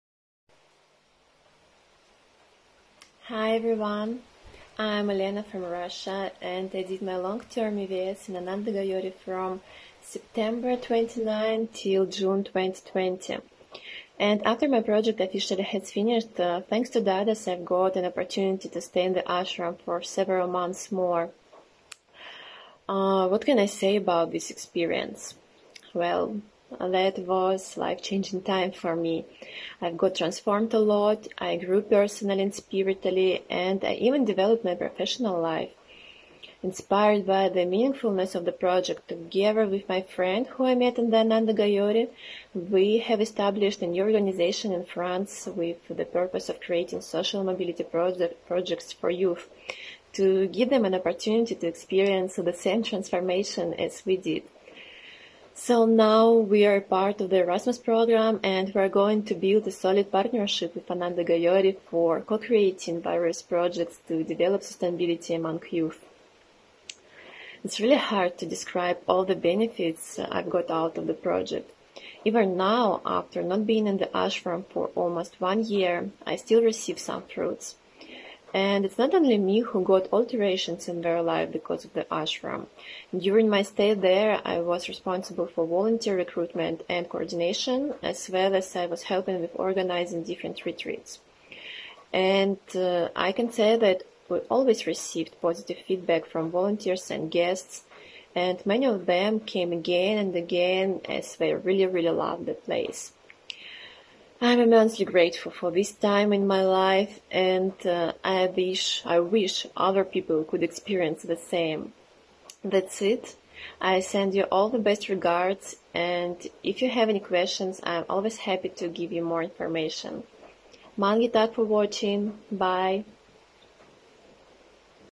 Volunteering Experiences of Elena from Russia
Elena shares how she benefitted from  being a European Solidarity Corps Volunteer with our "Sustainable Living Lab" project at Ananda Gaorii. Elena stayed on after her ESC volunteering period was over and helped recruiting, welcoming and supporting new volunteers along with many other useful services such as teaching yoga classes.